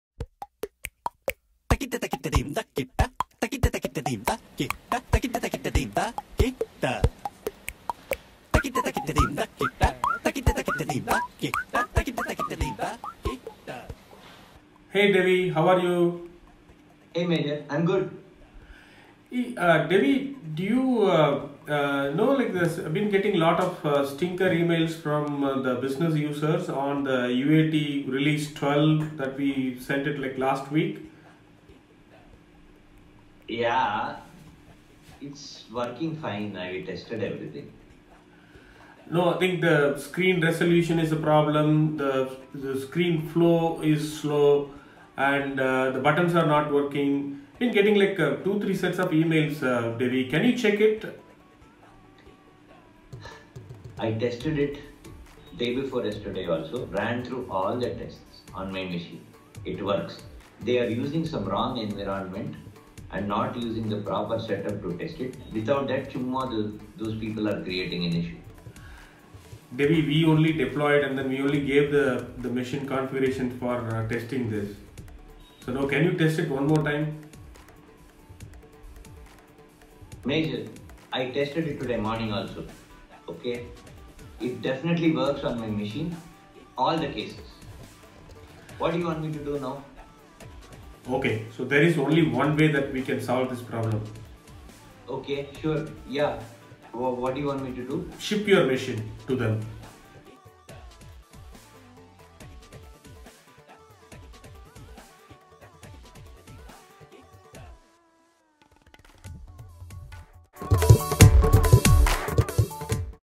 Tech Tortures - It works for me E6
The changes deployed do not work for the customer. Developer insists that it is working for him. What will the Project manager do?